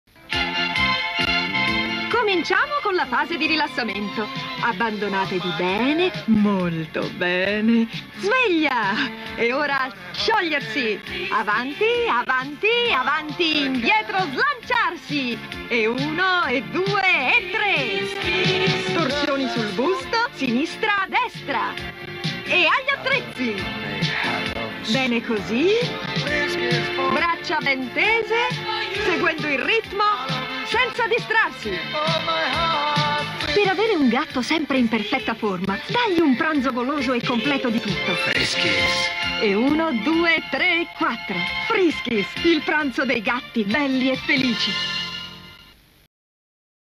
Spot - Crocchette FRISKIES - 1988 😺 (45 sec. HD)
Pubblicità anni 80 del cibo per gatti FRISKIES crocchette